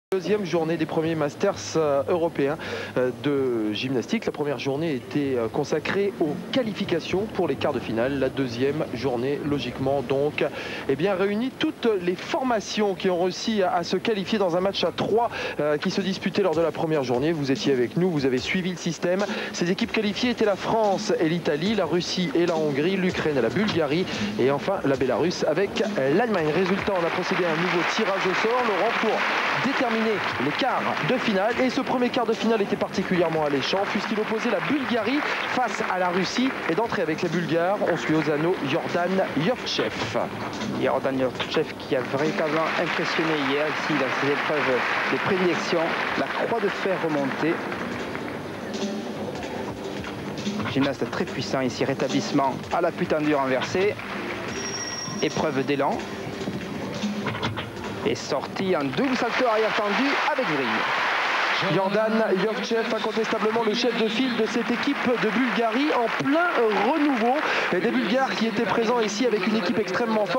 Jordan JOVTCHEV (BUL) rings - 1997 European masters Quarter final
Jordan JOVTCHEV (BUL)

'97 European Masters in Paris / Euromasters Quarter final